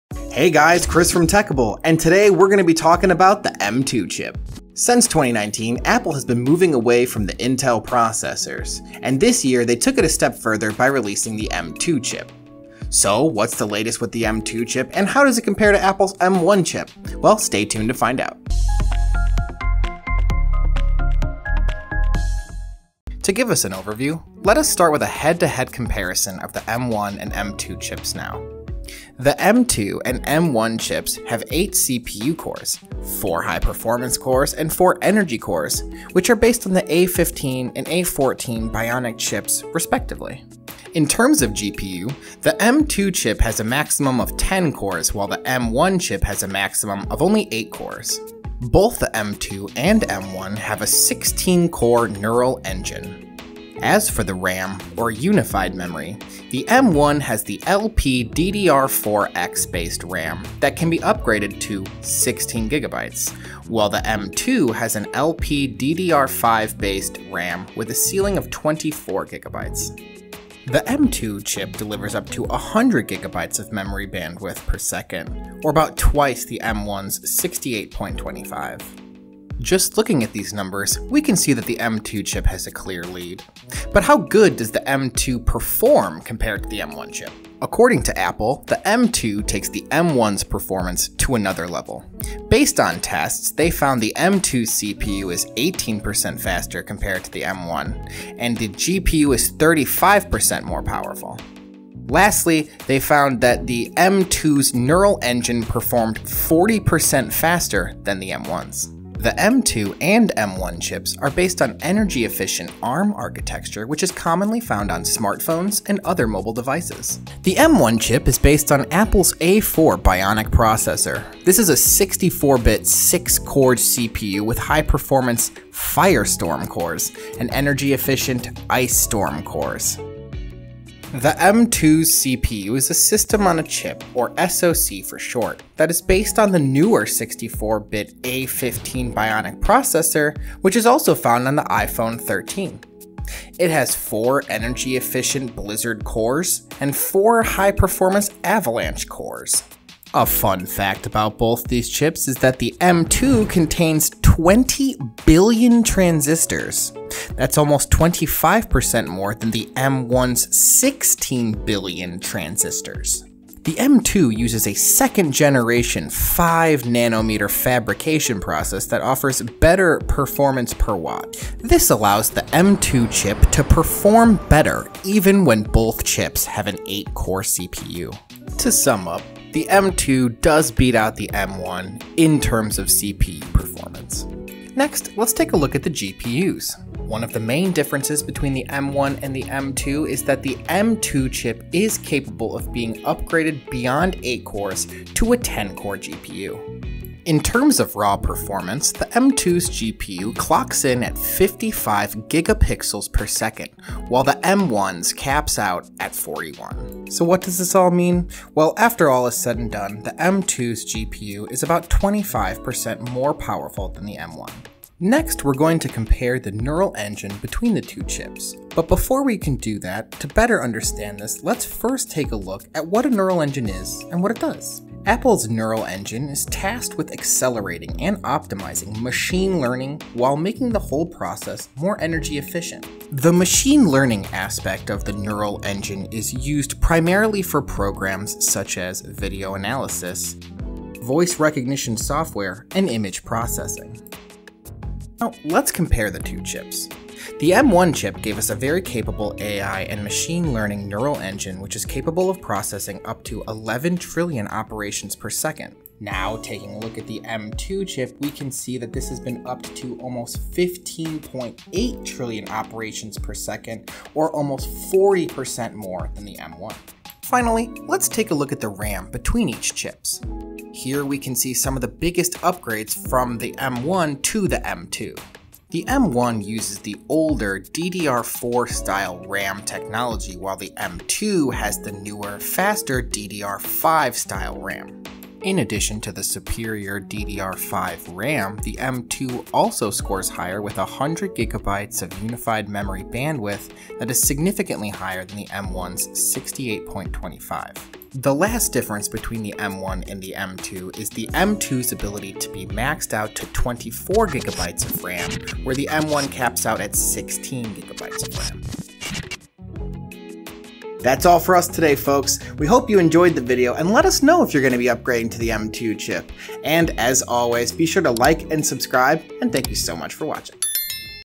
Apple M1 Vs M2 Chip - The Tech Explained
Apple has updated its MacBook Pro with its new M2 processor. It’s an improvement over the older M1 chip, especially in single-core performance. But the M2 will also power Apple’s cheaper MacBook Air, which will have a brand-new design and many other benefits. Here's a technical breakdown of Apple's M1 VS M2 Chips and what's improved.

 Save $100s on your next Mac at Techable.com